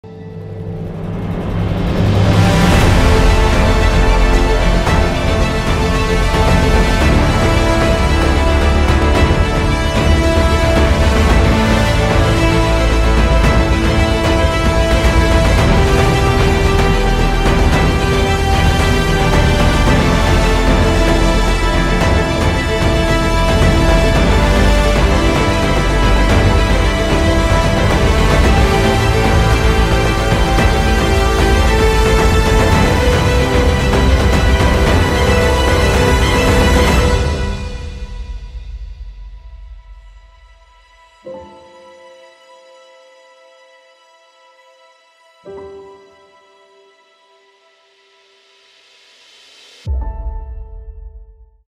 YOU CANNOT MISS THIS: CFC-18 Brazil 8 th of March | Centurion FC
The 2nd Event of the year, CFC18, will be live again from the beautiful Rio De Janeiro, with another stacked night of MMA and combat entertainment.

CFC continua avançando e estaremos de volta em março! Vamos que vamos! CFC 18 Brazil